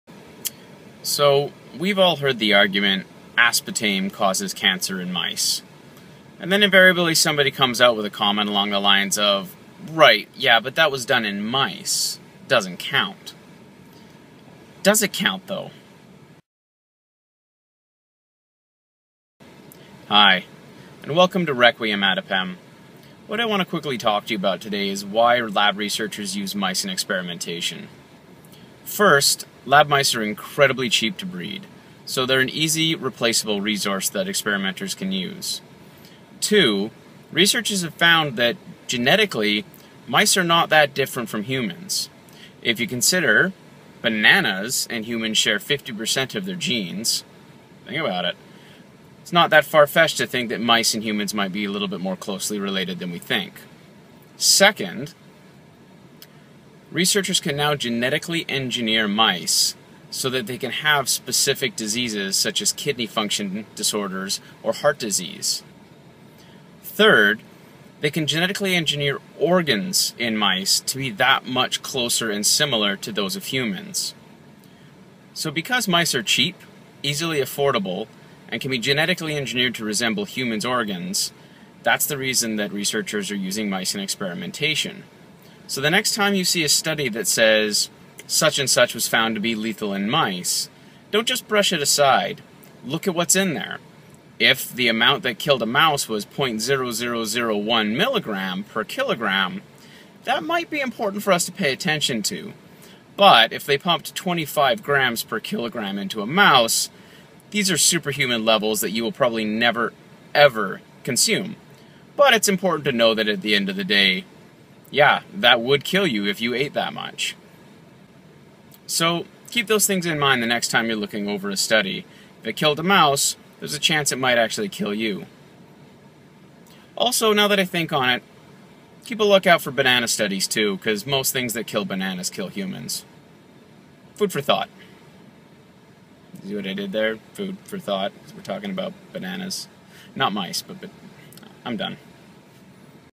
Why are mice used in lab experiments?
So we've all heard the argument that aspartame causes cancer in mice, then invariably someone comes out with the comment along the lines of "that was done in mice, it doesn't count". Does it count though? Requiem Adipem talks about why lab researchers use lab mice in experimentation. Cost, genetic similarities, and genetic engineering are all reasons mice are favoured by researchers. So next time experimental data is released on mice make sure you take a minute to closely examine, it could save your life!